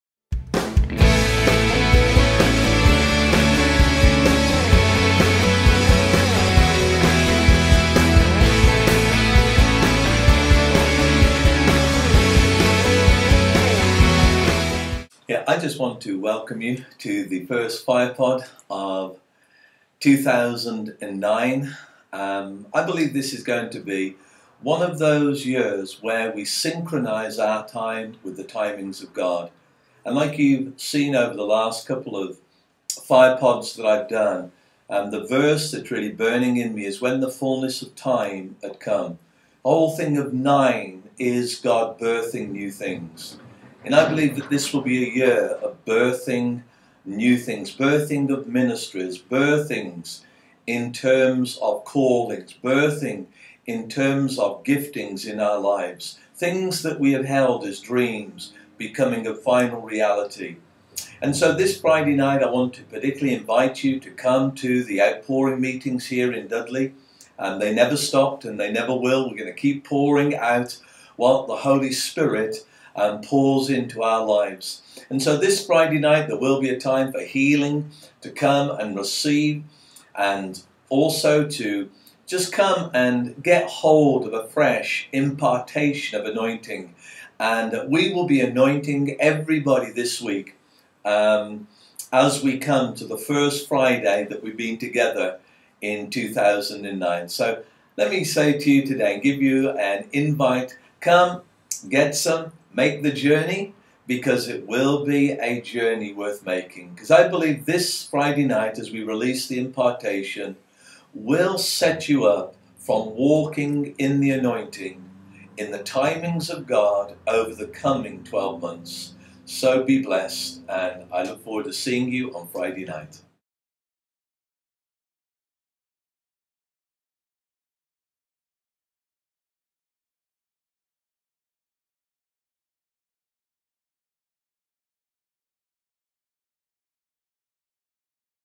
A Personal Invitation to the First Friday Night Meeting of 2009 by Trevor Baker
You are personal invited to our first Friday night meeting of 2009. We believe that this coming year is the year of God's timeline. So why not make the journey and be with us. It's a journey worth making.